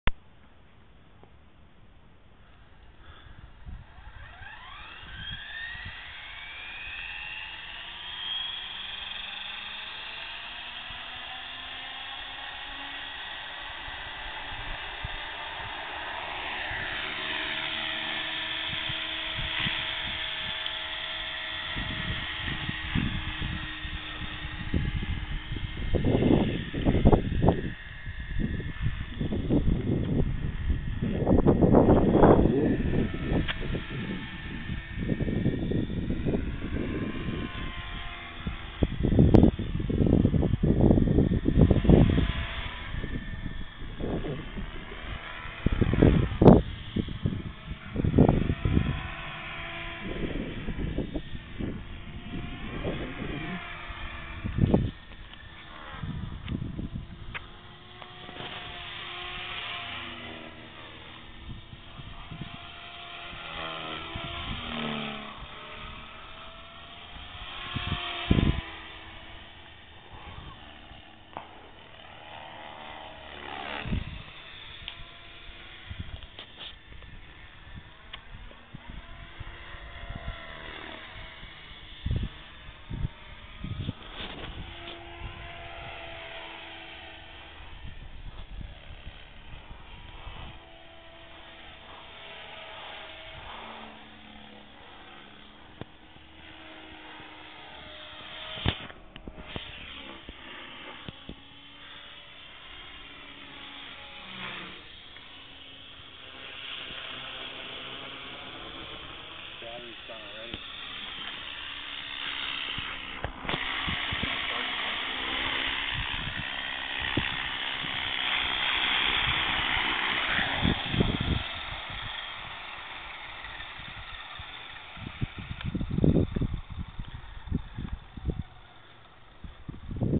align 450 3d pro
this little bugger is pretty damn fast for a 450 .dx6i high speed servos.